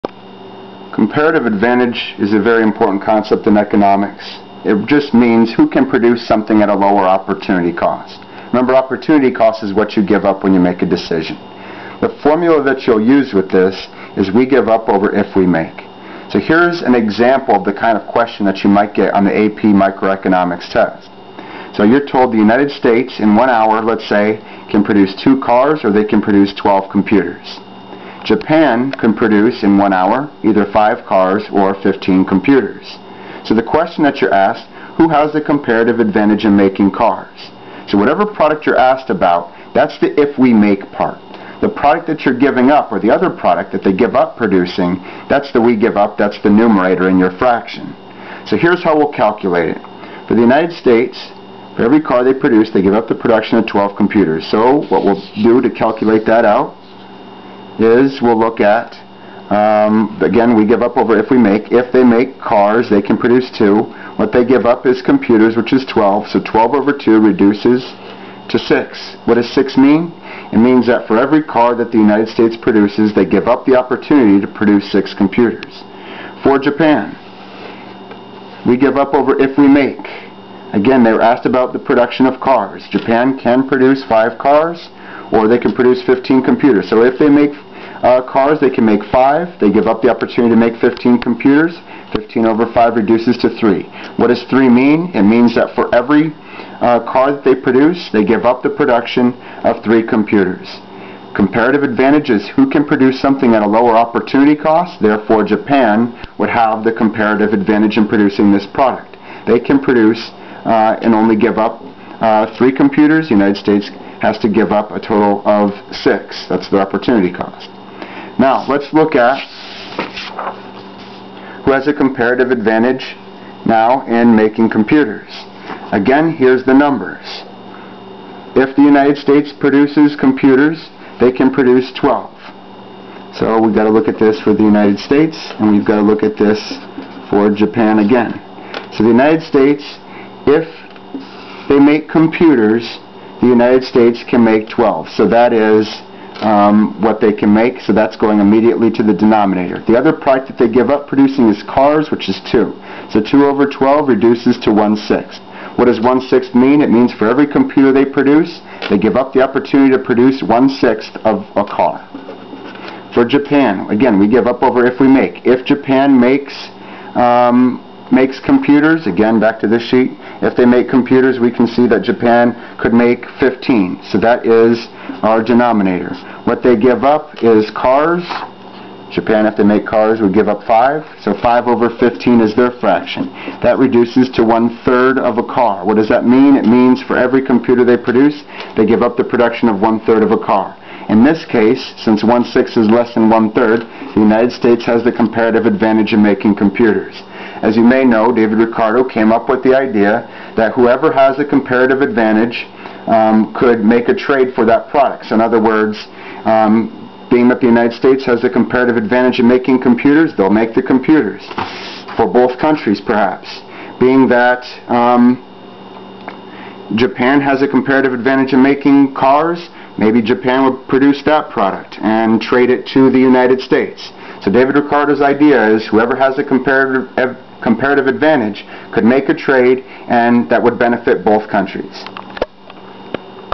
Comparative Advantage; How to Calculate it. Economics, AP Microeconomics
"AP" is owned by the College Board which does not endorse this site or the above review.

QUESTIONS:

1) In one hour, the USA can produce 3 cars or 12 computers.
In one hour, Japan can produce 2 cars or 14 computers.

a) Which country has the comparative advantage in making cars?
b) Which country has the comparative advantage in making computers?
c) What kind of trade should there be between the two countries? In other words, what should they specialize in making?